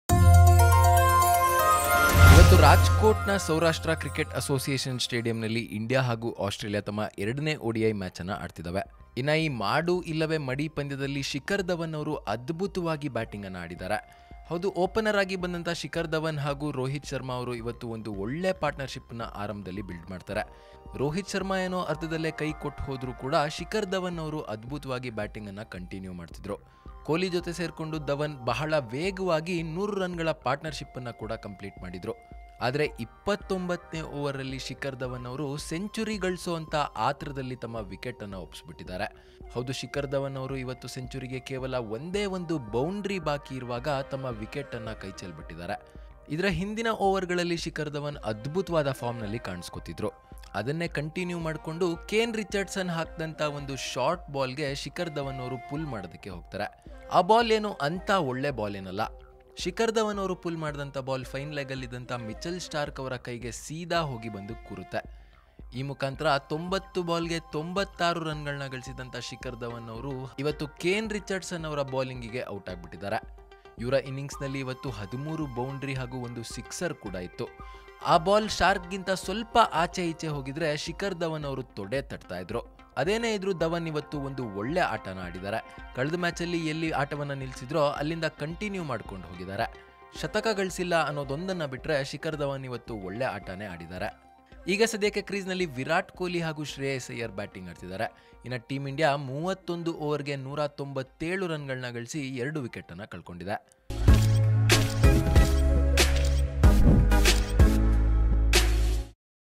IND vs AUS 2nd ODI : Shikhar loses his calm at 96 | SHIKAR DHAWAN | INDIA | RAJKOT|ONEINDIA KANNADA
ರಾಜ್‌ಕೋಟ್‌ನ ಸೌರಾಷ್ಟ್ರ ಕ್ರಿಕೆಟ್‌ ಅಸೋಸಿಯೇಷನ್ ಸ್ಟೇಡಿಯಂನಲ್ಲಿ ಇಂದು (ಜನವರಿ 17) ಎರಡನೇ ಏಕದಿನ ಪಂದ್ಯಕ್ಕಾಗಿ ಪ್ರವಾಸಿ ಆಸ್ಟ್ರೇಲಿಯಾ ಮತ್ತು ಆತಿಥೇಯ ಭಾರತ ಮೈದಾನಕ್ಕಿಳಿದಿದೆ. ಟಾಸ್ ಗೆದ್ದಿರುವ ಆಸ್ಟ್ರೇಲಿಯಾ ಬೌಲಿಂಗ್‌ ಆಯ್ದುಕೊಂಡಿದೆ.

 India and Australia are playing their second ODI match at Rajkot today and India is trying to keep the series alive. Shikhar continues from where he left in the previous match. but loses his calm at 96
Oneindia kannada